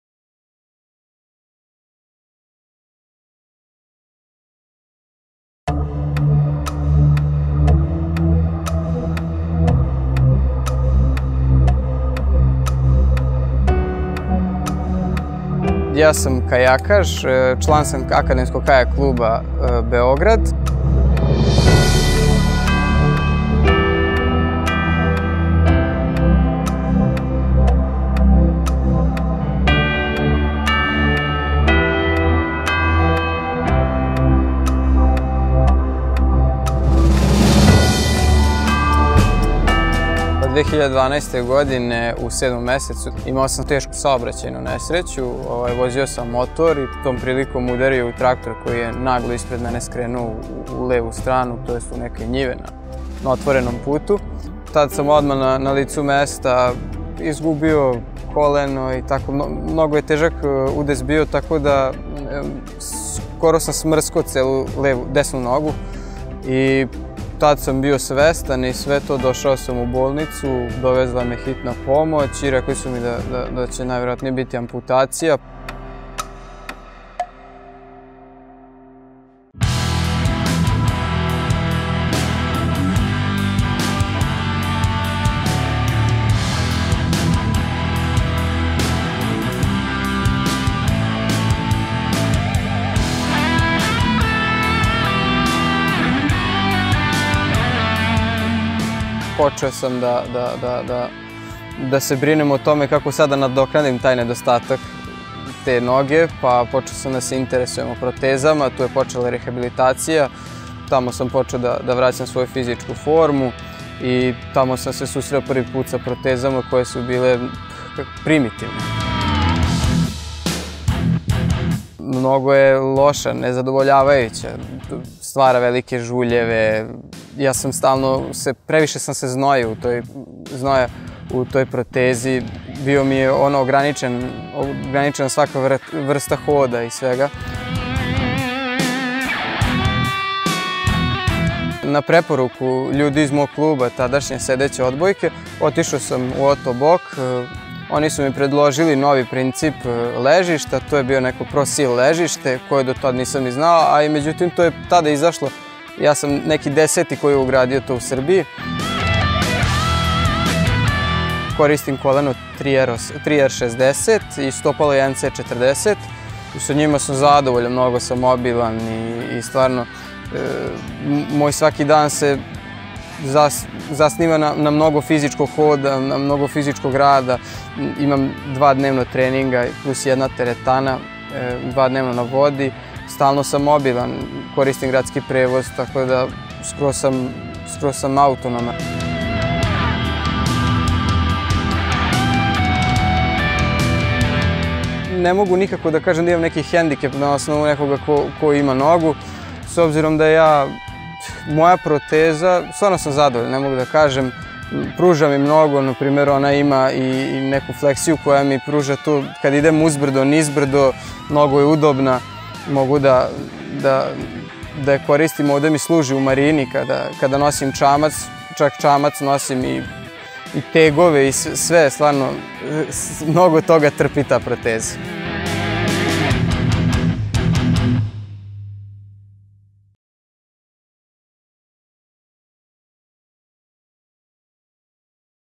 Ottobock - Arsen Arsenović, Novi Sad
Arsen Arsenović (22) uspešno studira na Elektrotehničkom fakultetu Univerzita u Beogradu. U pauzama učenja vredno radi sa svojim roditeljima na velikom poljoprivrednom imanju u rodnom Kulpinu. Svakodnevno trenira tri puta dnevno - nabija snagu u teretani i vesla na Dunavu, jer je član kajakaške reprezentacije Srbije. Arsen sve to postiže iako od 2012. godine, nažalost, nema desnu nogu, koju je izgubio u teškoj saobraćajnoj nesreći. Ipak, zahvaljujući protezi Ottobock, kako sam kaže, sebe ne doživljava kao osobu sa hendikepom, jer je još aktivniji nego pre zlosrećnog događaja...